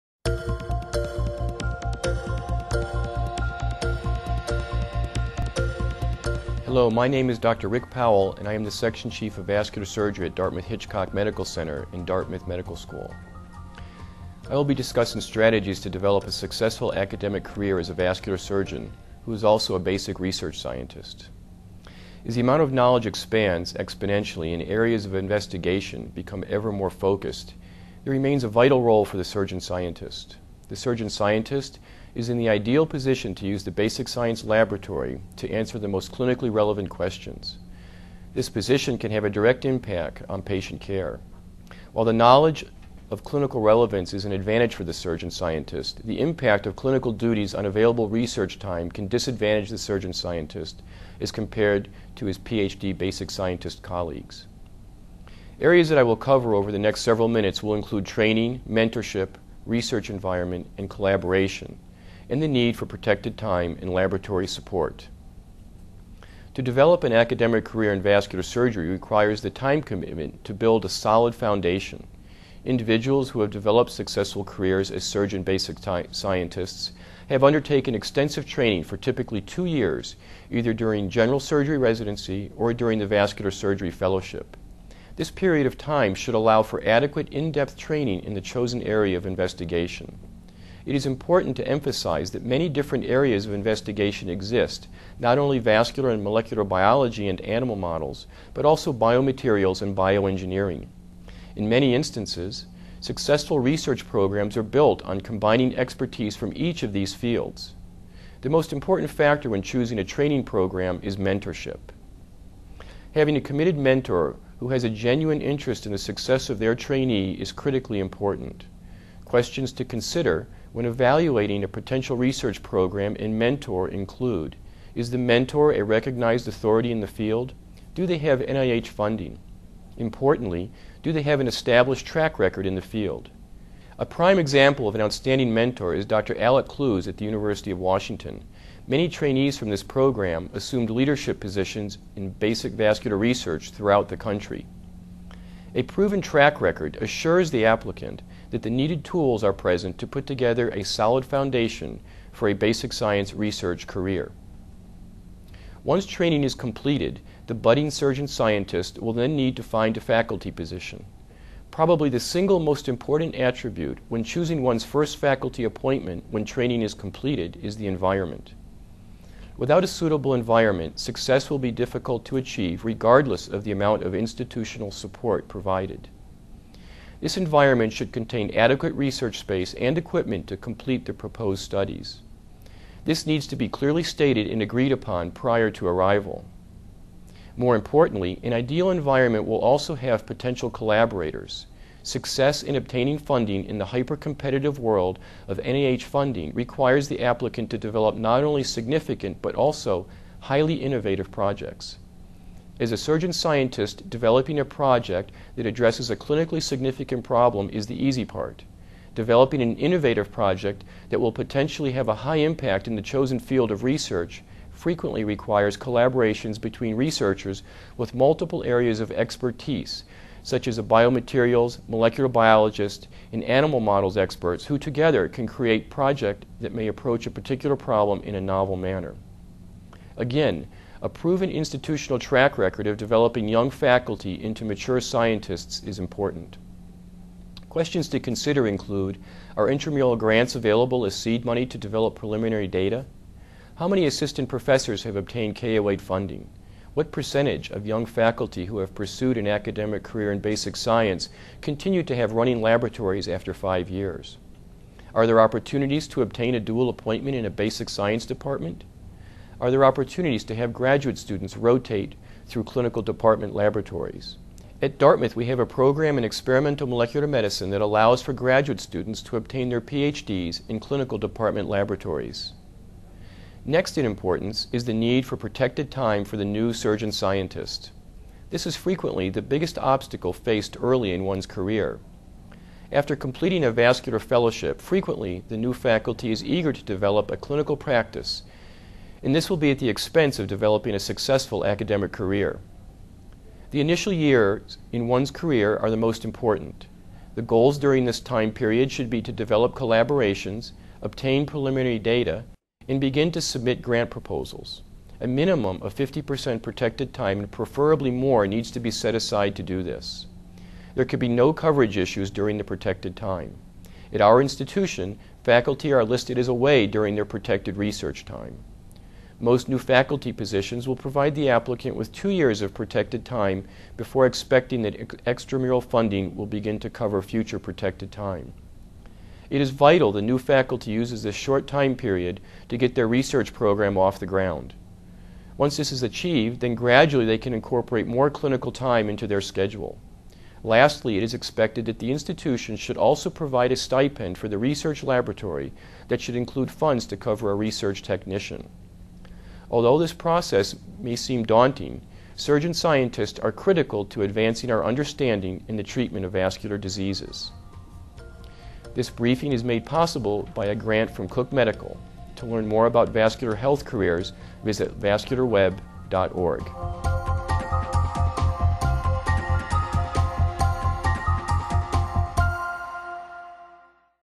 How Do I Get Started in Basic Research?
Hello, my name is Rick Powell and I am the section chief of vascular surgery at Dartmouth Hitchcock medical center and Dartmouth medical school.  I will be discussing strategies to develop a successful academic career as a vascular surgeon who is also a basic research scientist.